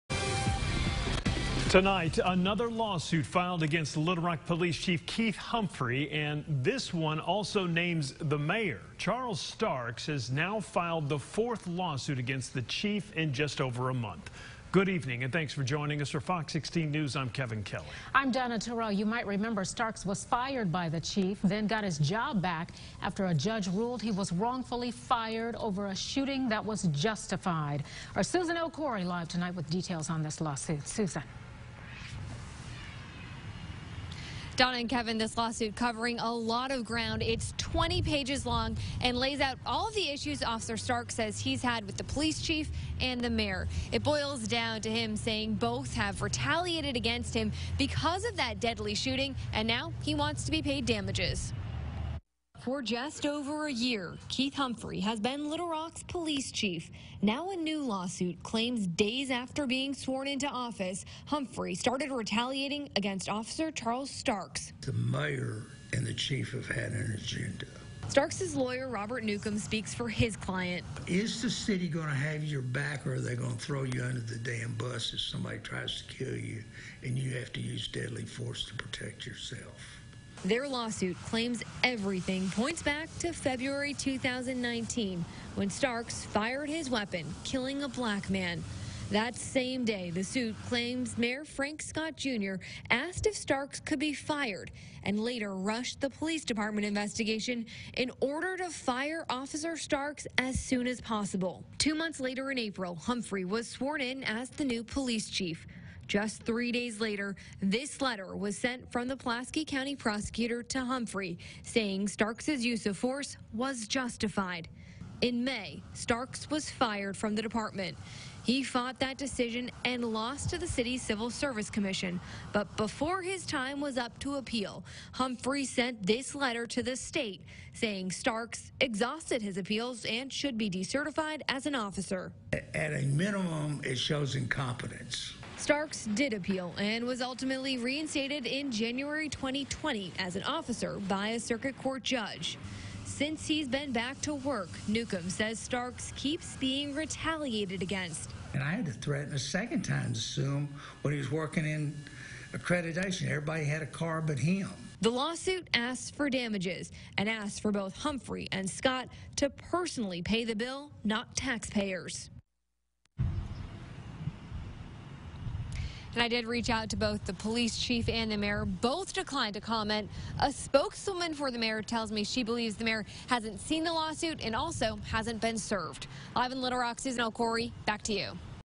LRPD officer involved in deadly use of force shooting suing police chief, mayor
A Little Rock Police officer who a judge ruled was wrongly fired by the city, is now suing Chief Keith Humphrey and Mayor Frank Scott, Jr.